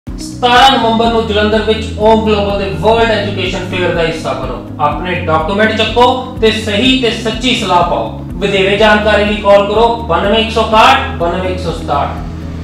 17 November Jalandhar World Education Fair 2019 || Canada - Australia - UK
Save the Date 17 November
Mega World Education Fair 2019
For Canada - UK - Australia by Aum Global
Pay Fee After Visa

Date - 17 November Sunday

Venue - Opp. Residency Hotel Near Bus Stand Jalandhar